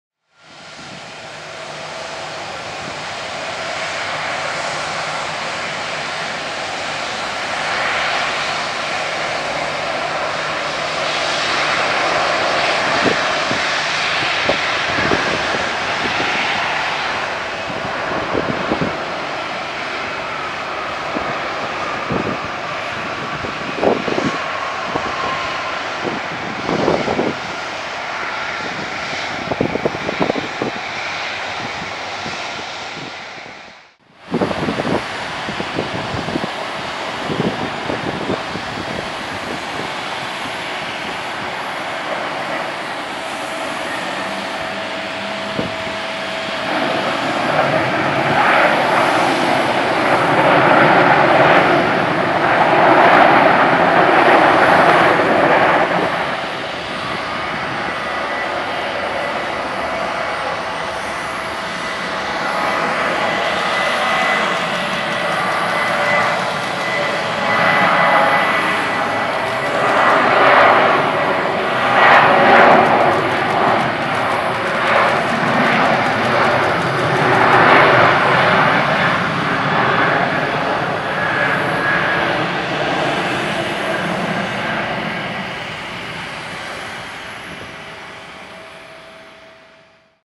Spotting Houston KIAH 3-12-15
Here we have a few clips while waiting on Qatar One World. United 772 Star Aliance and the BBJ SAS were nice to see. First time I've seen the SAS 737 as well.